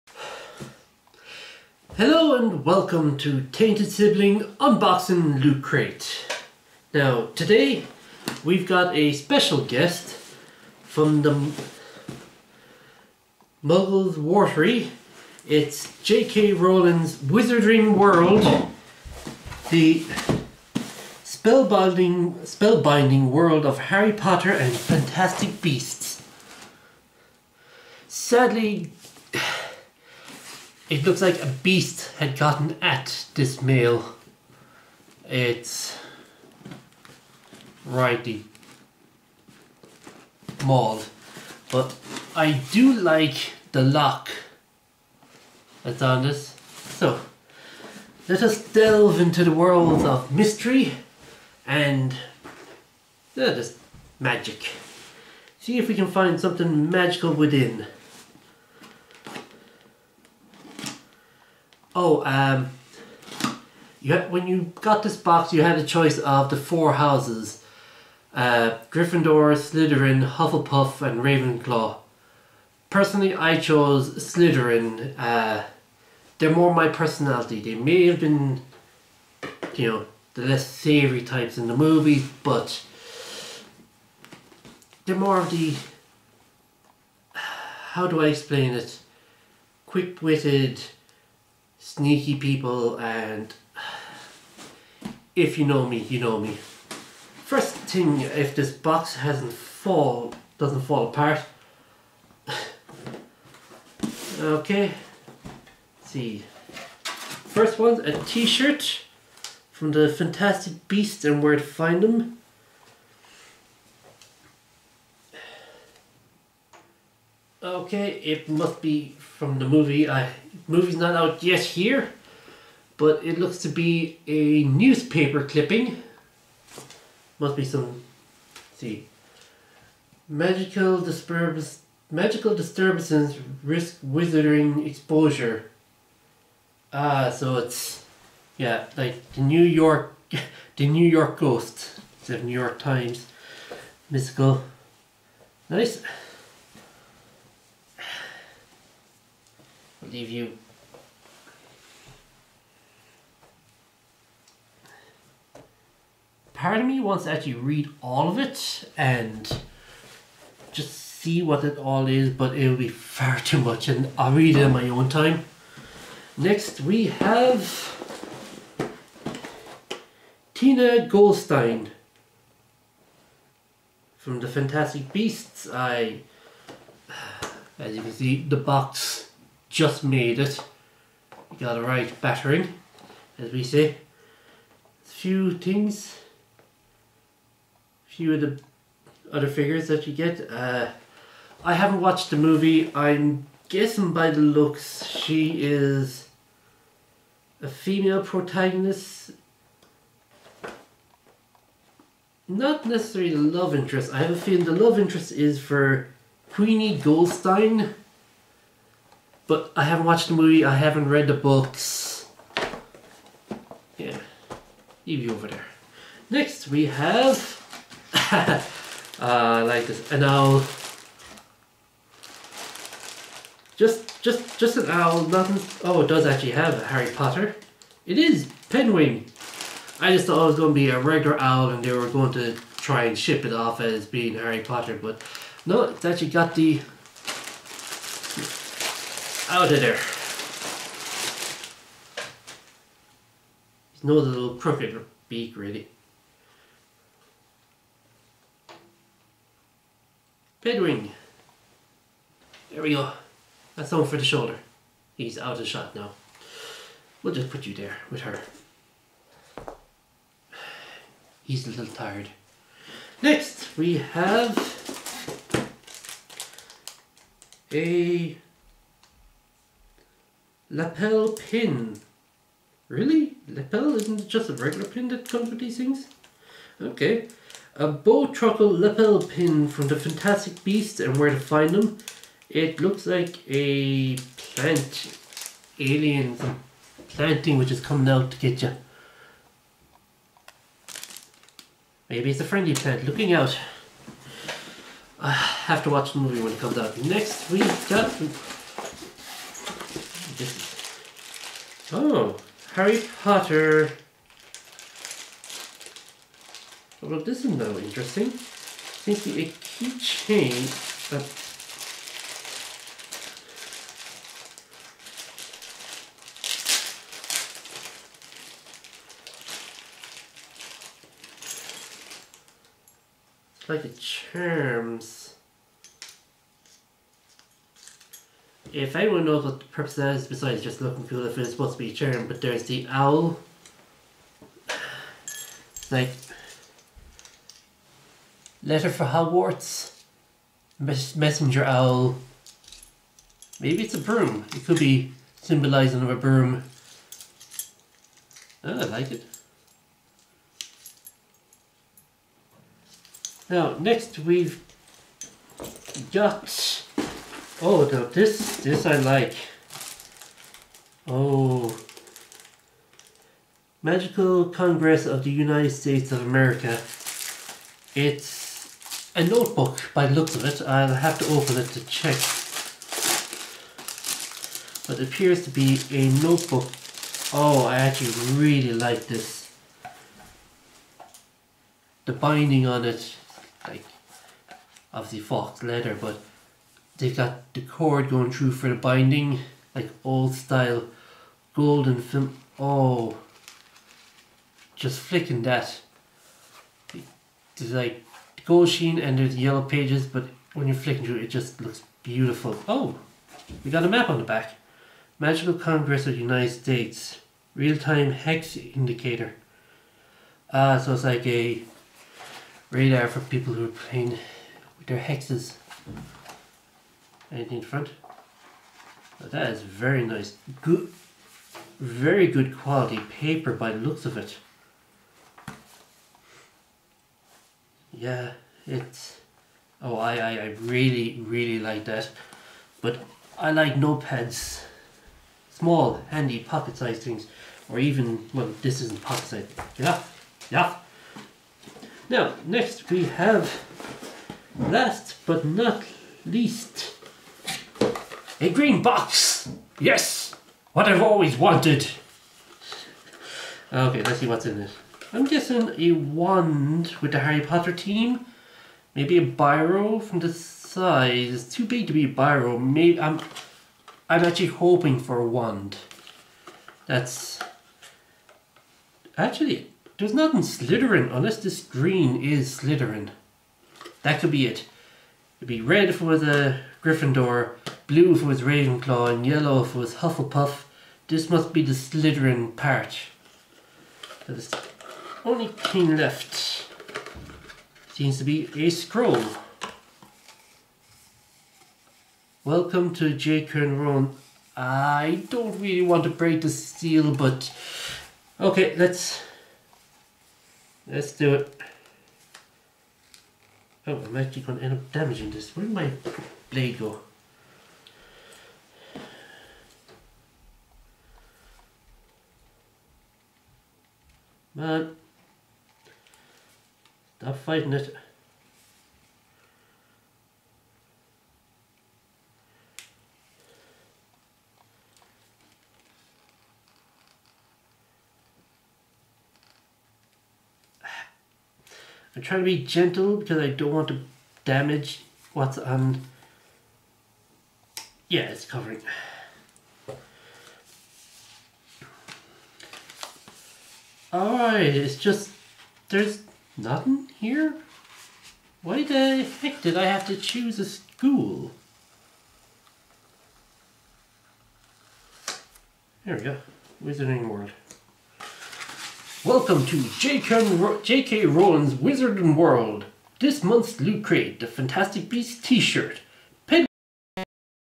Loot Crate JKRowling's Magical World November | My Magical Experience Begins
Loot Crate - JKRowling's Magical World - November

Loot
Fantastic Beasts T-Shirt
Pigwidgeon Plush
Pop! Figure - Tina Goldstein
Bow Truckle Pin
Hedwig/Letter Keychain
Macusa Notebook

And A Green BOX!